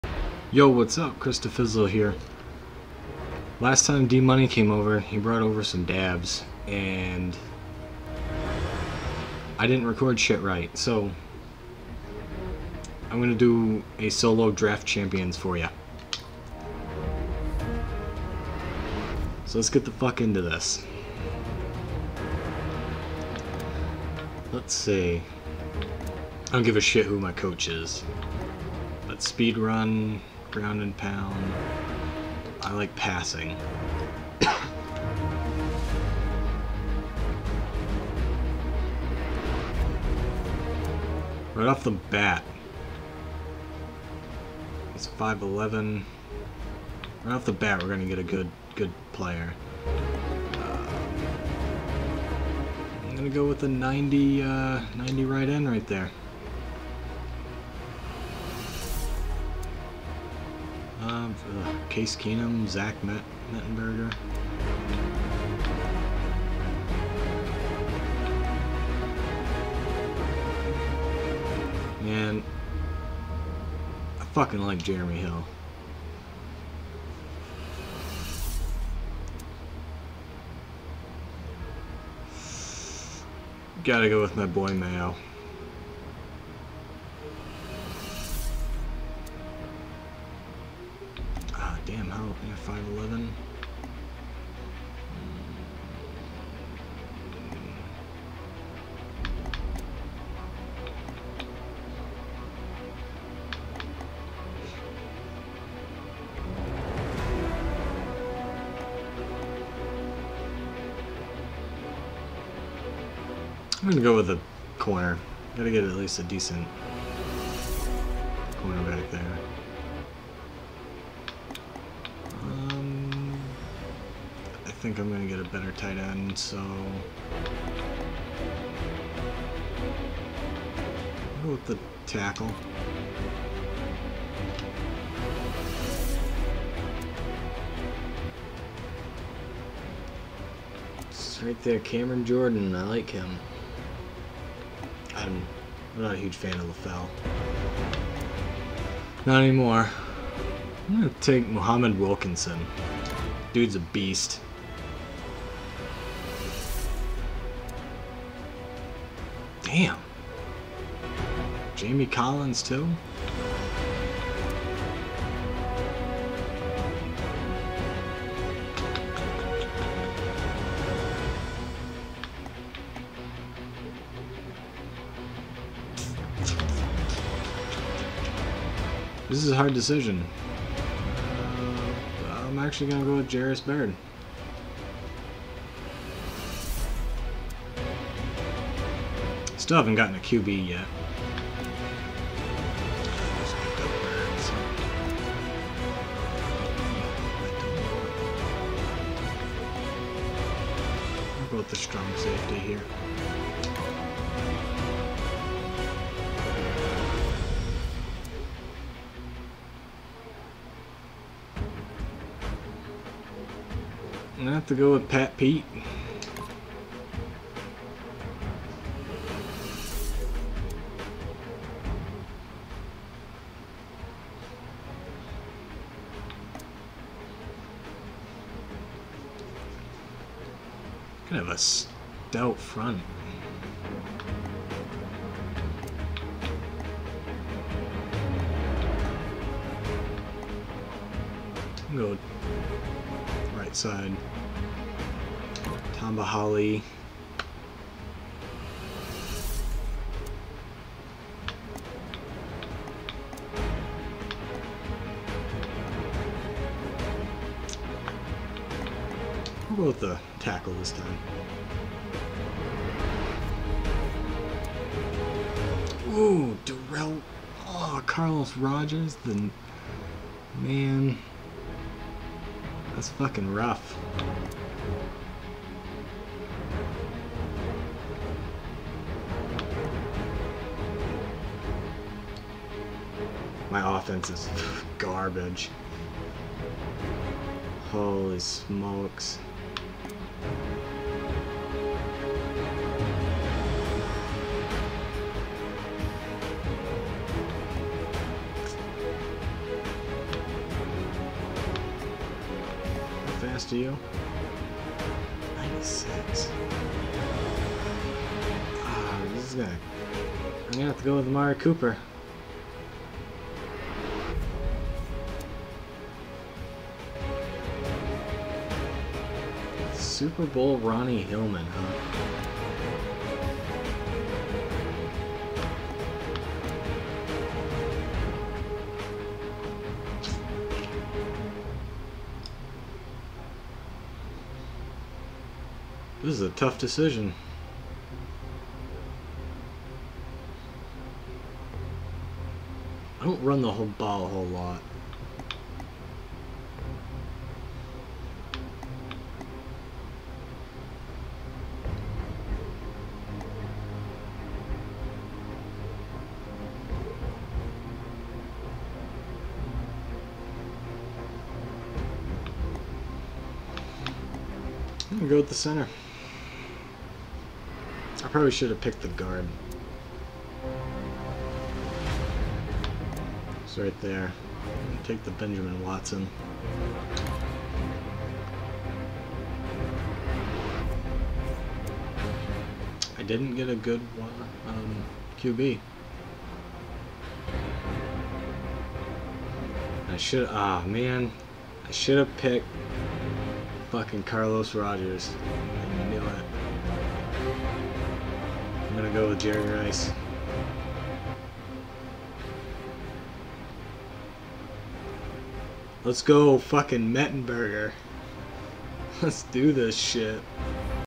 Road to a Draft Champion! Let's Draft this effin team!
So I fucked up all the recordings D$ and I did the last time. So I bring you some solo draft champions. Game 1 soon to come but for now let's get the fuck into this 15 round draft!